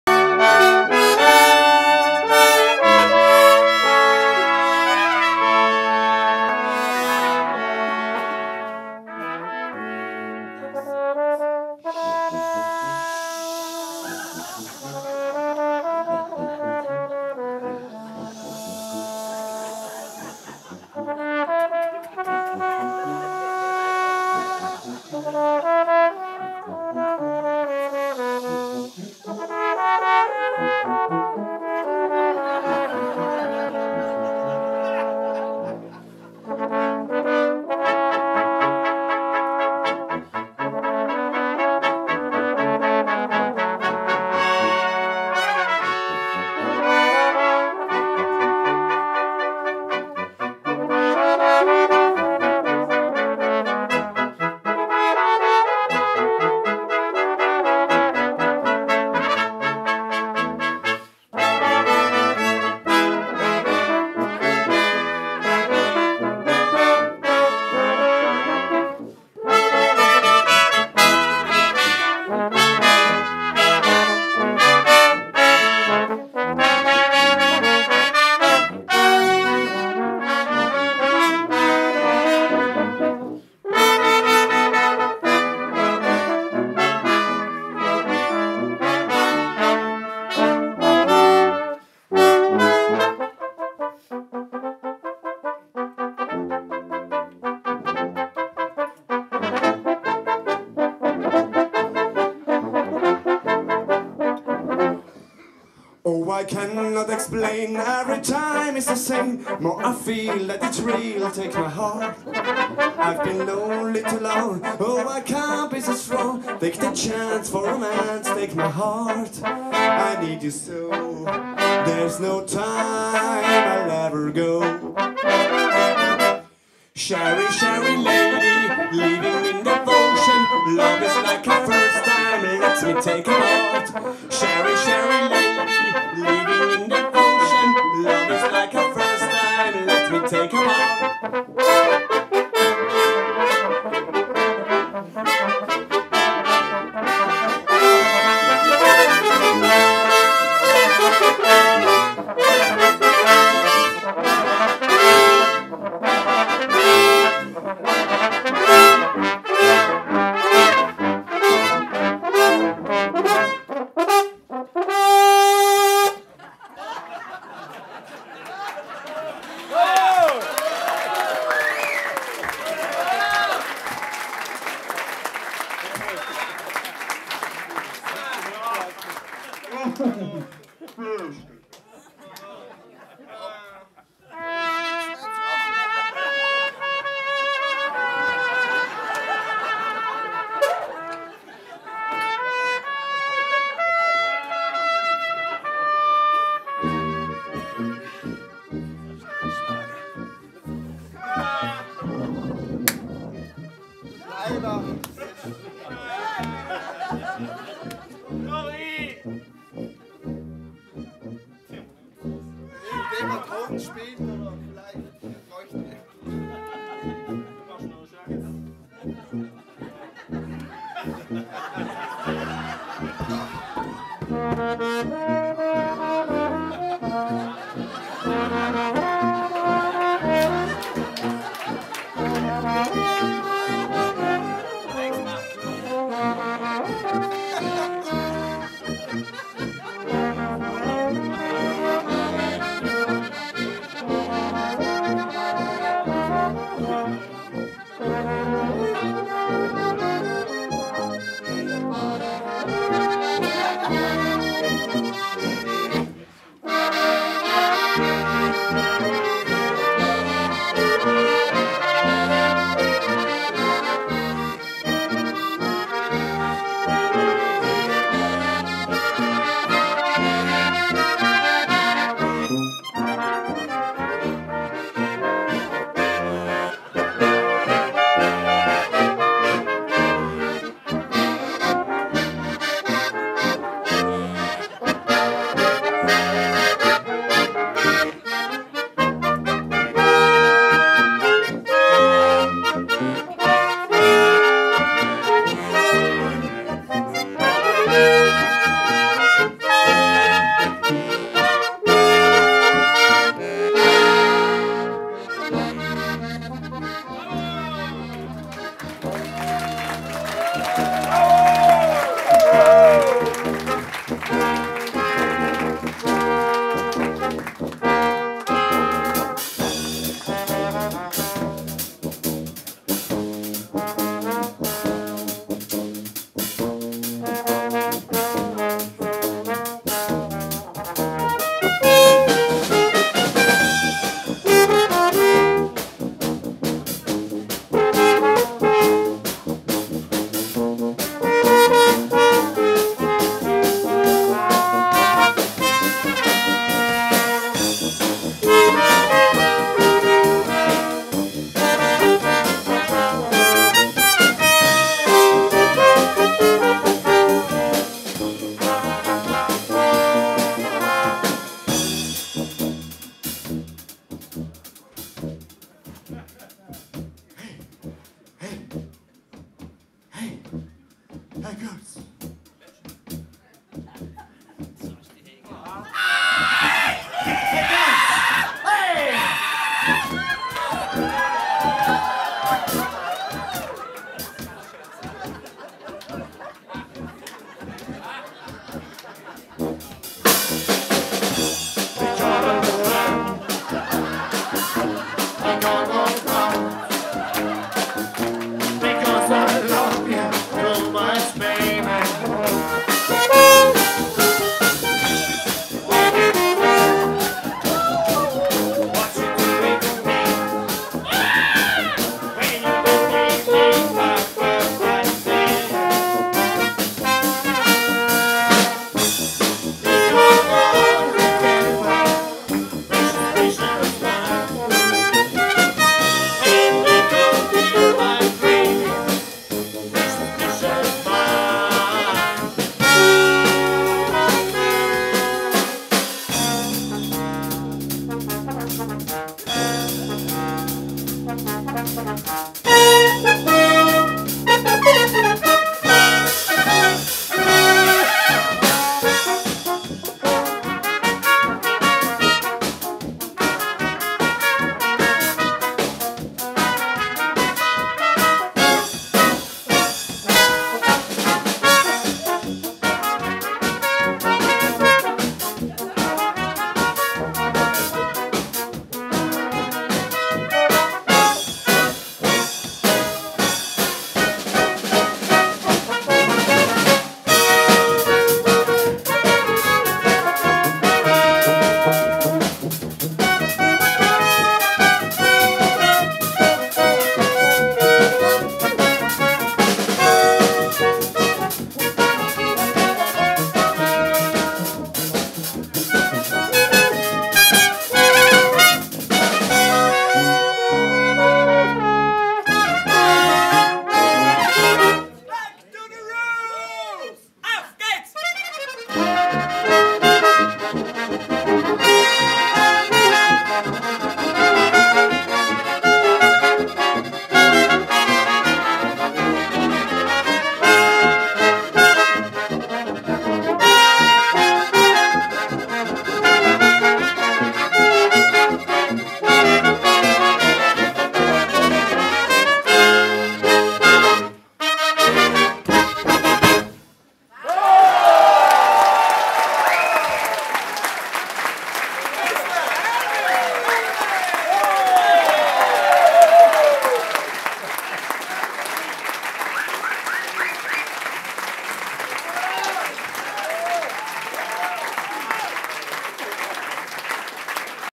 Oldies but Goldies - Bozen Brass live @ Musik beim Wirt, Fornach
Bozen Brass spielten am 24.3.18 im Wirtshaus Lohninger, Fornach, Österreich
Anton Ludwig Wilhalm, Trompete, Gesang
Manuel Goller, Trompete, Gesang,
Robert Neumair, diverse Instrumente, Gesang
Norbert Fink, Horn, Gesang
Martin Psaier, Posaune, Gitarre, Gesang
Anton Pichler, Tuba, Gesang